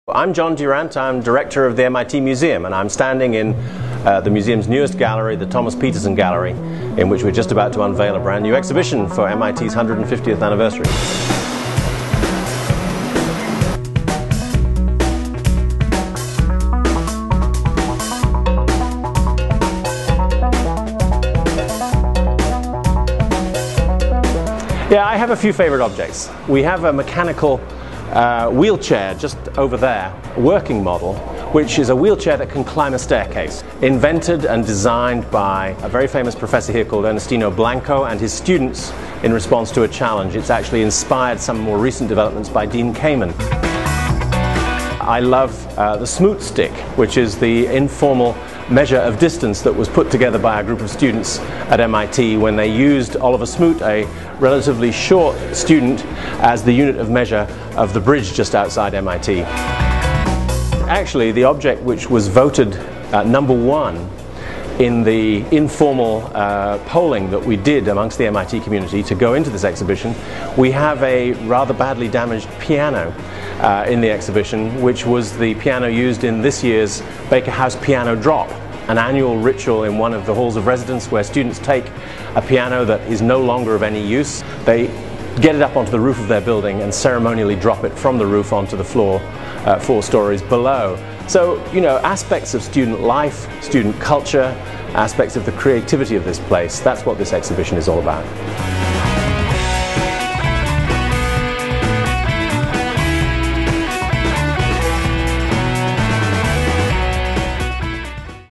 MIT 150: World-Changing & Weird Innovation
To mark MIT's 150th anniversary, the MIT Museum exhibits 150 objects -- many of them voted in by the community -- that tell the story of world-changing and quirky innovation over the years. The exhibit opens to the general public Saturday, Jan. 8, 2011.